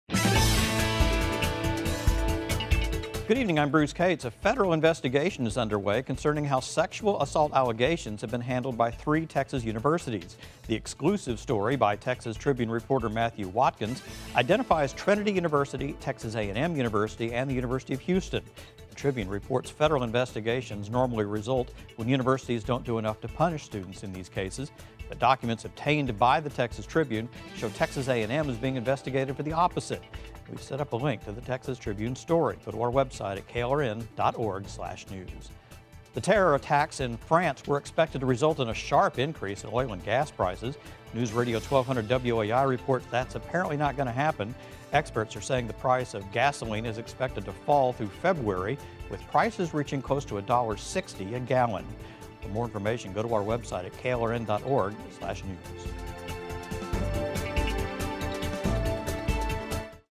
Tuesday, Nov. 17, 2015 - Update 2
Topics: Officials Investigate Universities, Terror Not Impacting Oil Prices

A federal investigation is under way over how sexual assault allegations have been handled by three Texas Universities.

The exclusive story by Texas Tribune reporter Matthew Watkins identifies Trinity University, Texas A&M University and the University of Houston as the targets of the investigation.

The Tribune reports federal investigations normally result when universities don’t do enough to punish students accused of rape or harassment.

But, in documents obtained by the Texas Tribune, Texas A&M is being investigated for the opposite.

We’ve set up a link to the Texas Tribune story.

The terror attacks in France were expected to result in a sharp increase in oil and gas prices.

News Radio 1200 WOAI reports that is apparently not going to happen.

Experts are saying the price of gasoline is expected to fall through February with prices reaching close to a $1.60 a gallon.